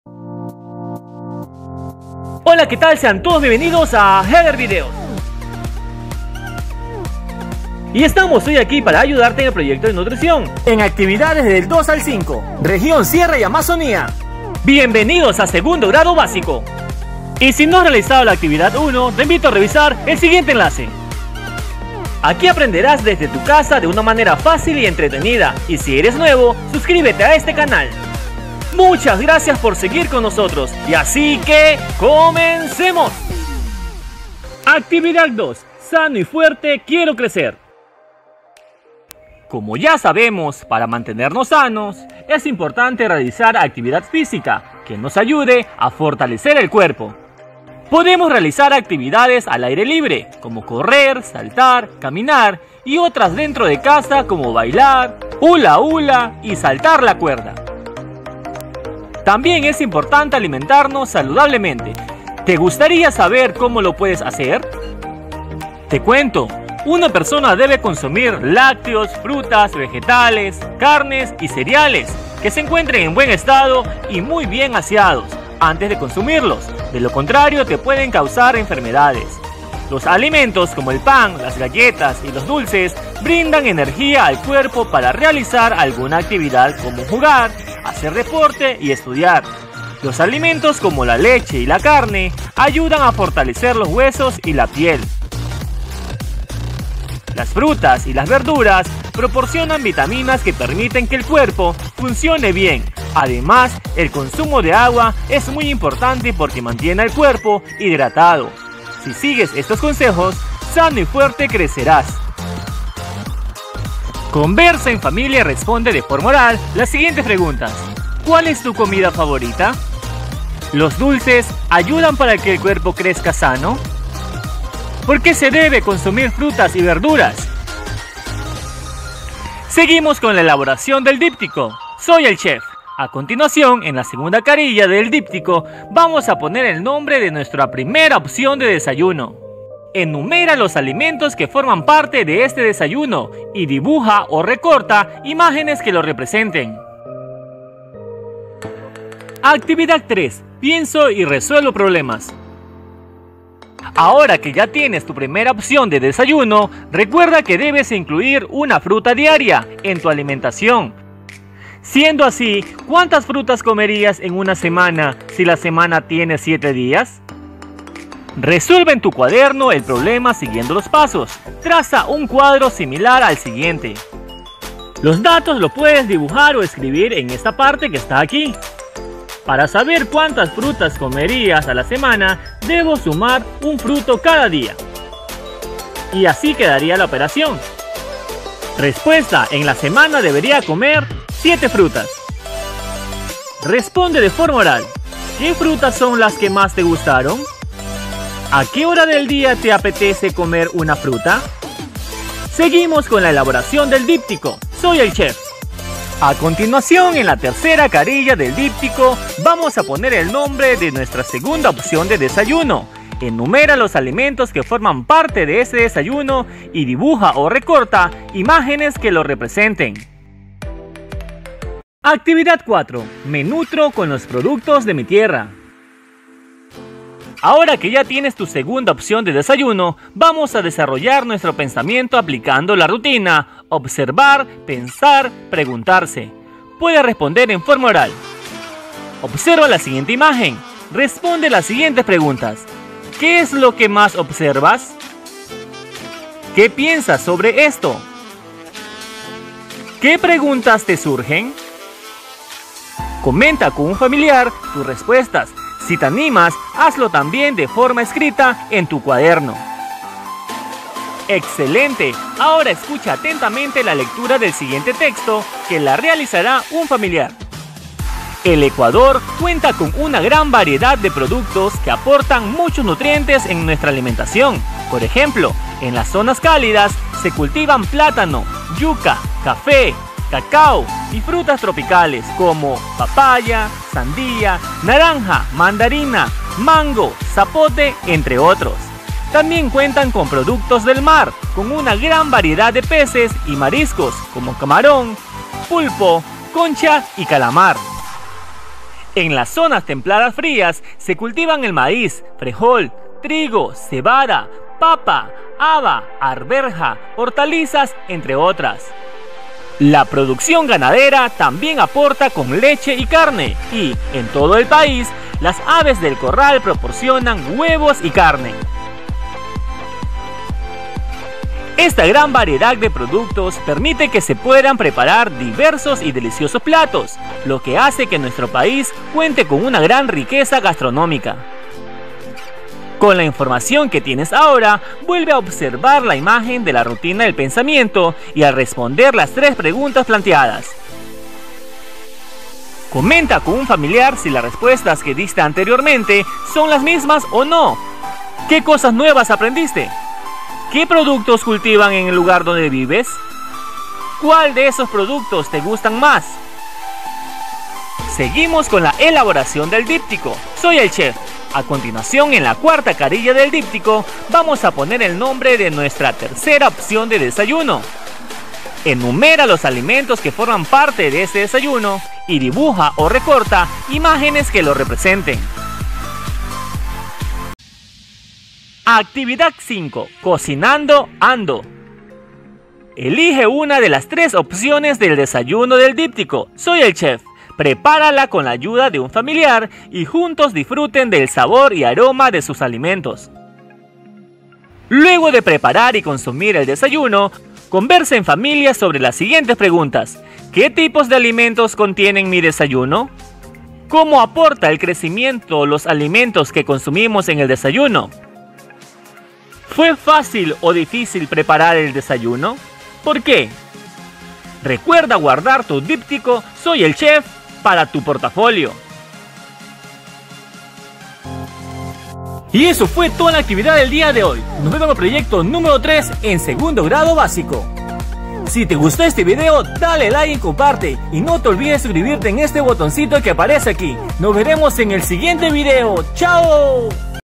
PROYECTO DE NUTRICIÓN. SEGUNDO GRADO ACTIVIDAD 3, 4 Y 5. SIERRA Y AMAZONIA.
PROYECTO DE NUTRICIÓN SEGUNDO GRADO PARTE 2
SOY EL CHEF
Actividad 2: Sano y fuerte quiero crecer
Actividad 3: Pienso y resuelvo problemas
Actividad 4: Me nutro con los productos de mi tierra
Actividad 5: Cocinando ando